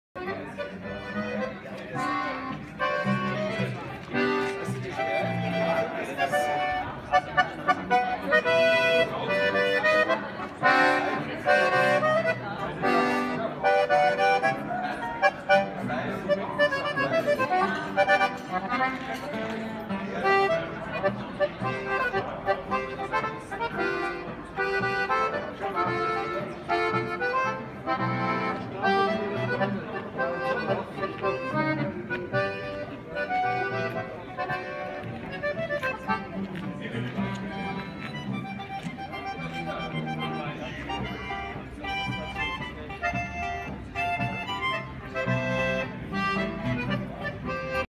Street music, Bad Toelz, Bavaria, Germany. July, 2017
Eisdiele Grangelato am Marktstrasse
Пешеходная улица в Бад Тёльце, Бавария, Германия. Июль, 2017
Уличные музыканты на выходе из магазинчика мороженого Гранжелато